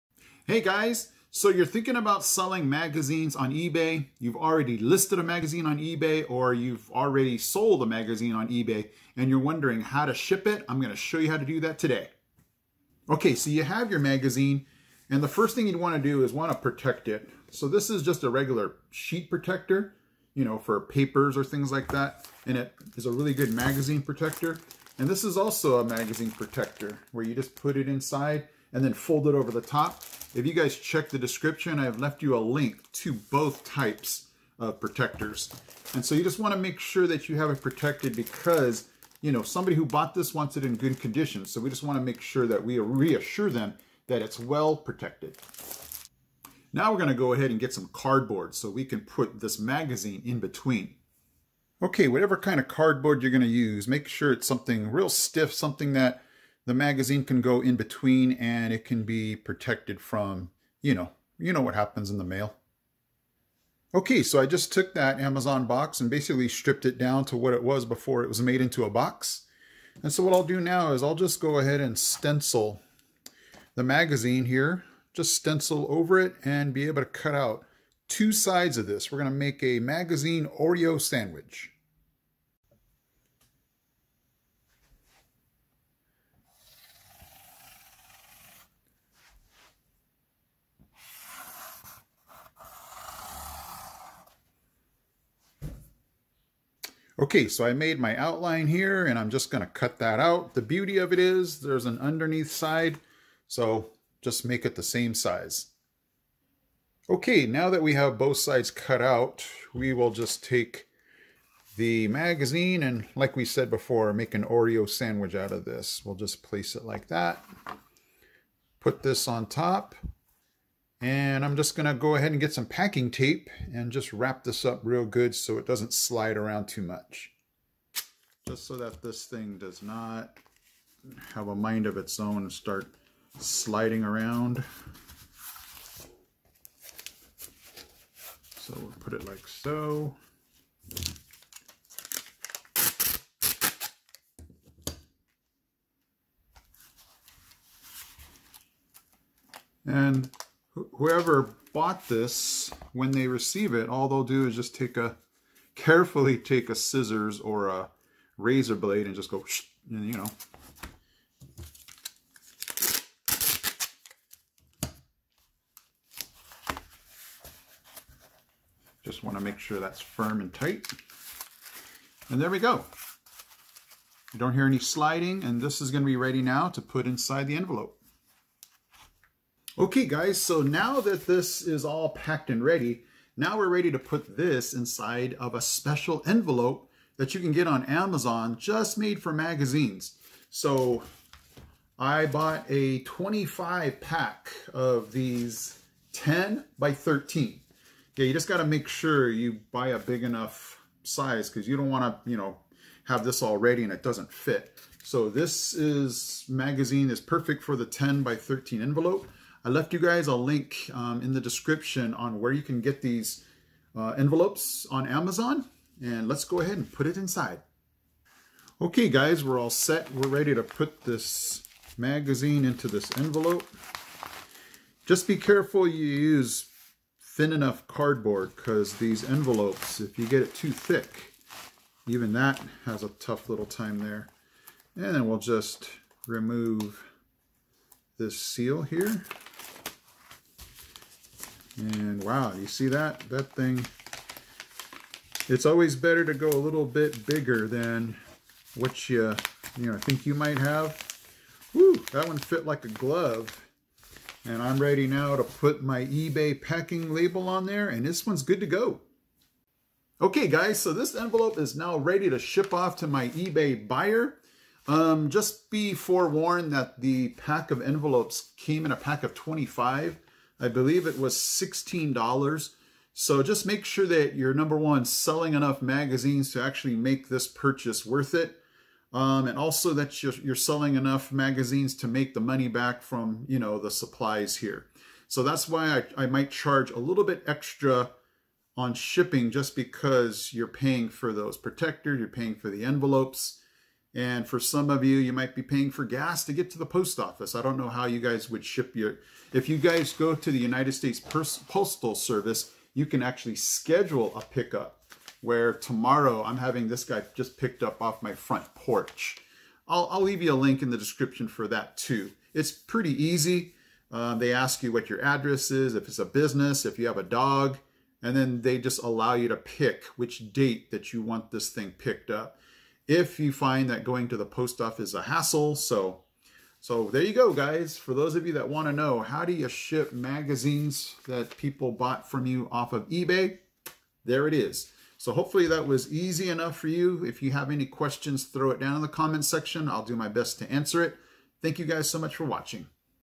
How To Package and Ship Magazines You Sold On Ebay
Hey guys!!  Whether you are planning to sell magazines on Ebay, you're currently selling magazines on Ebay, or you've already sold a magazine on Ebay, this video will show you how to package and ship that magazine!!  I will show you each step I took to package the magazine I just sold on Ebay. Below are the links to everything I used to package the magazine.

 BCW Crystal Clear 2-mil Polypropylene Magazine Bags 8-3/4" X 11-1/8" with 1-1/2" Flap. (100-Count)
Ultra PRO Life Magazine 11-5/8" x 15-1/8" Bags (100 Count Pack)
10 X 13 Magazine Bubble Envelopes
12 X 16 Magazine Bubble Envelopes